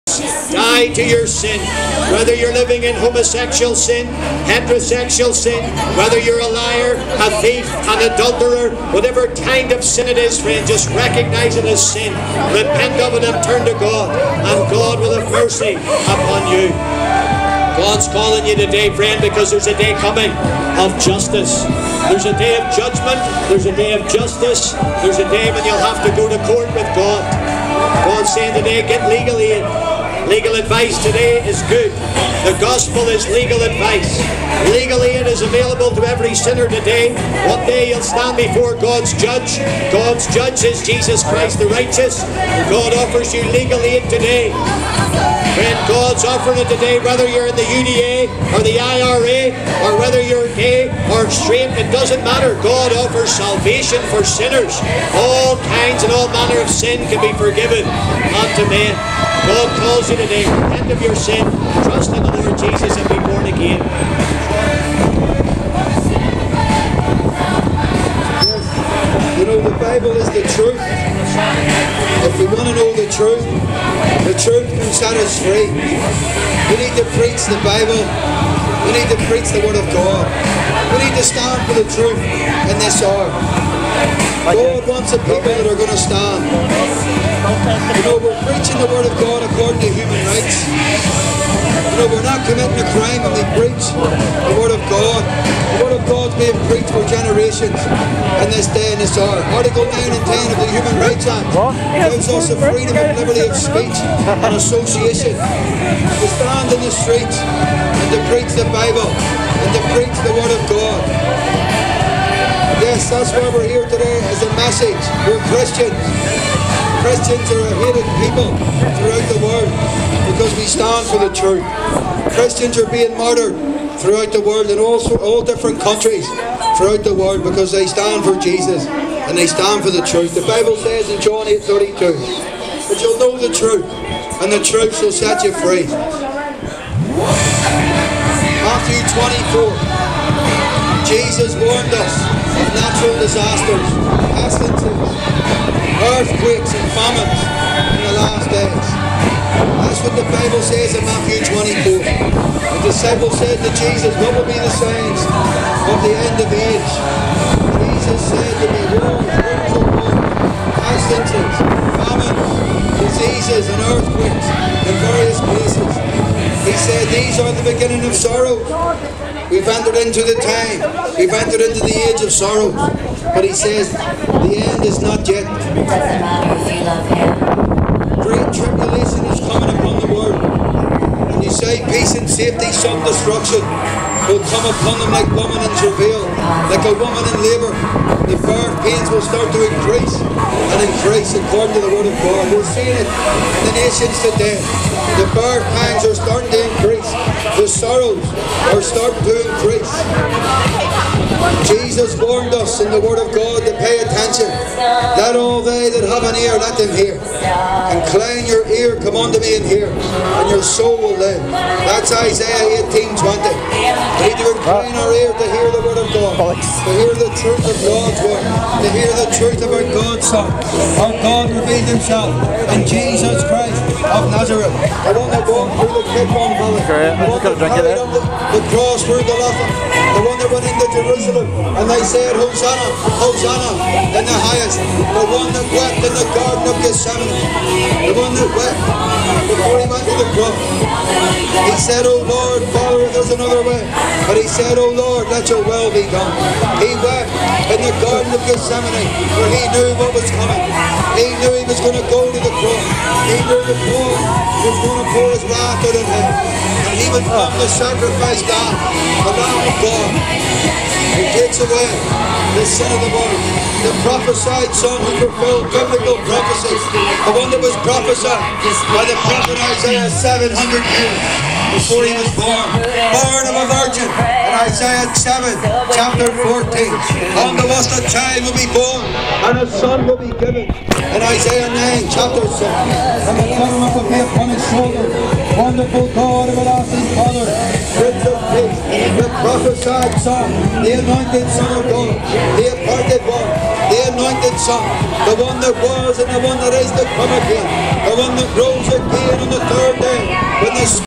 Belfasts favourite hate preachers back out again
preachers ryan and sean paul back out being anti LGBTQ+